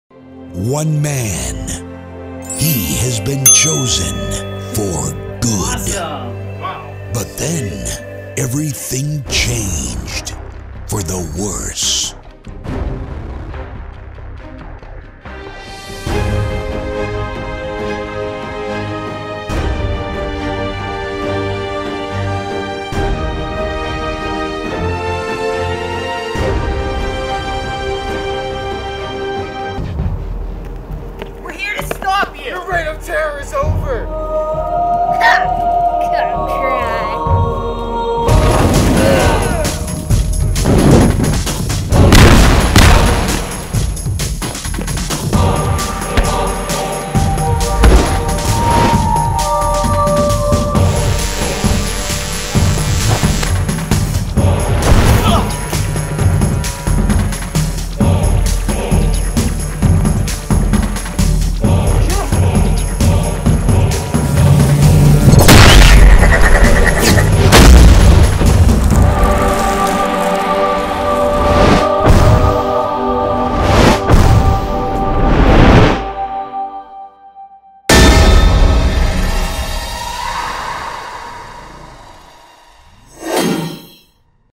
A Dark Evil, another epic battle
An Epic Fight Between Good and Evil. Once upon a time there was a young man who did only what was good, But then Everything changed. Two brave warriors Rise to challenge this dark evil ! Will they win?? Guess you have to watch the video to find out!

Credit for most Sound effects and Trailer Voice goes to Final Cut King